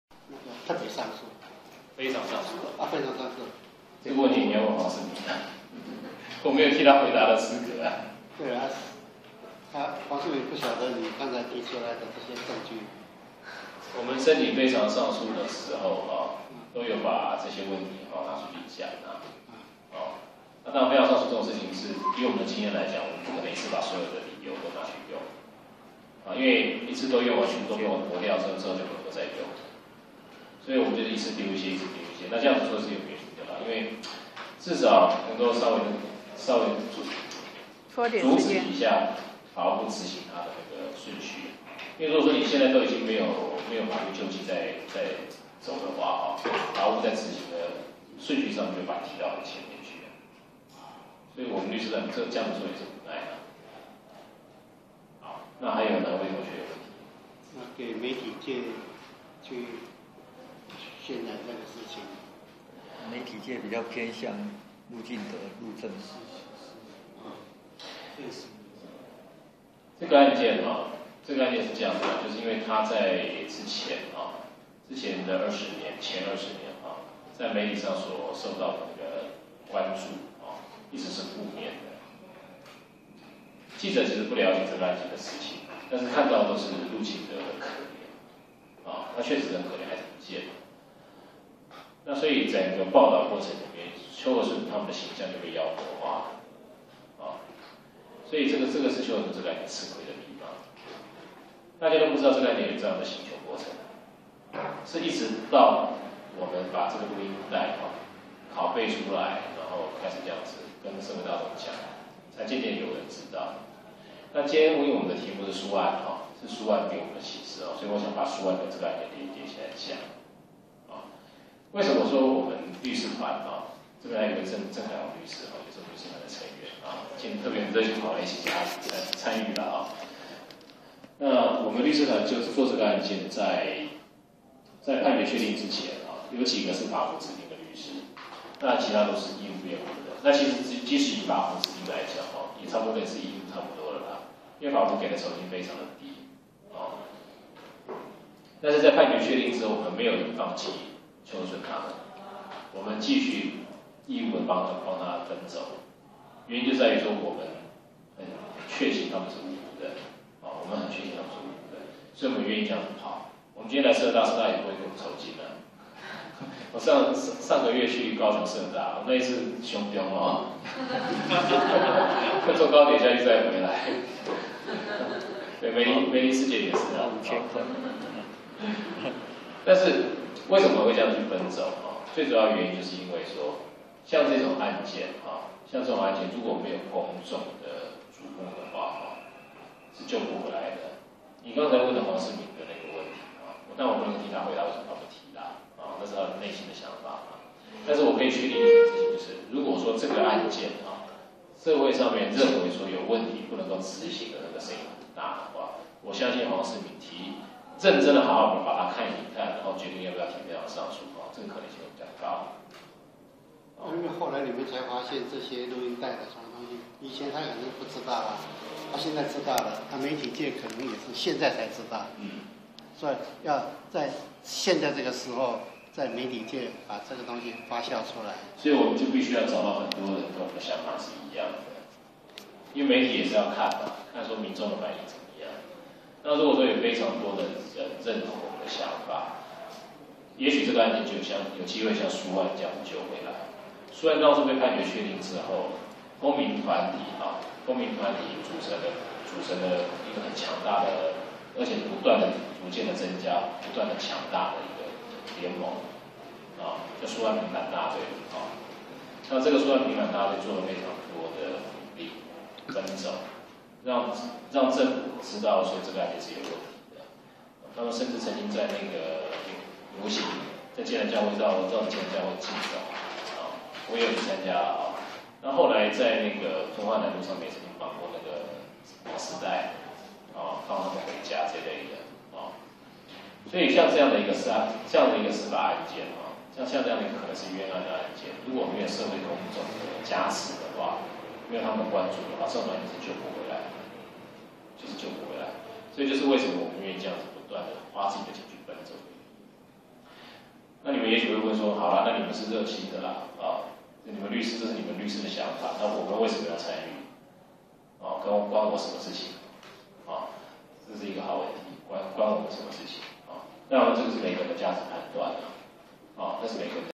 邱案沒有輿論的參與是就不回來的 蘇案就是有很多的公民大隊VIDEO0852
邱案訴訟與救援策略─ 救援邱和順志工律師尤伯祥 士林社大公民週